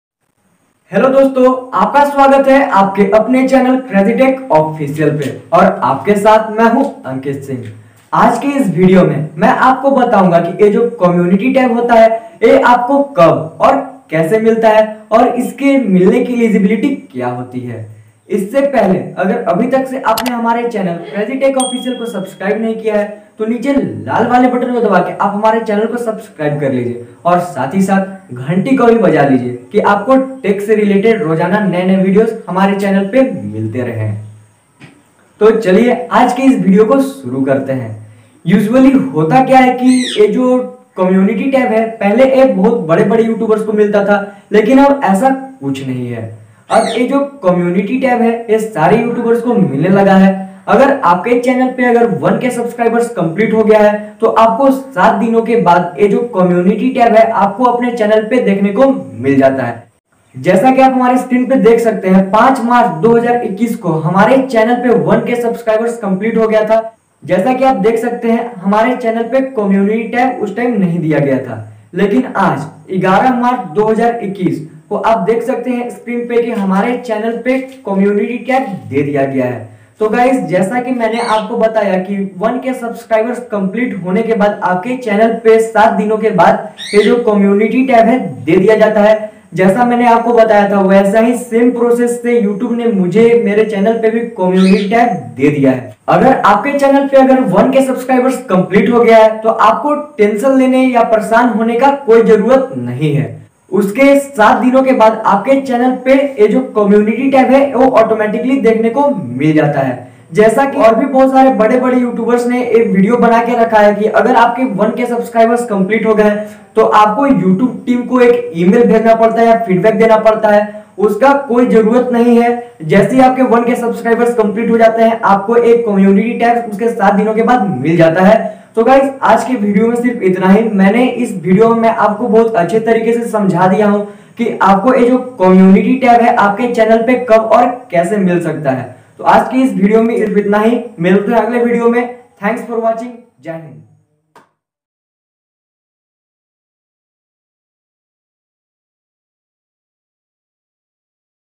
communitytab Community tab kab or kaise milta h | How do we get community tab on YouTube.
In this video I have showed you how enable community tab on YouTube.
Aapko  aapke  YouTube channel pe community tab kab or kaise milta h